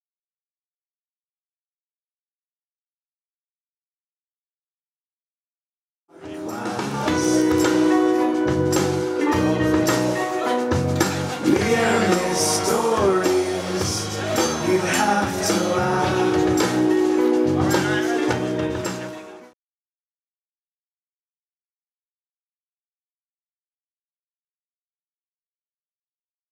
Immersive Ideal with Beauty Pill at Artisphere
A sublime installation of creativity and mind remixing.  Check out the Immersive Ideal with Beauty Pill at Artisphere in Rosslyn, VA through Jan. 22, 2012.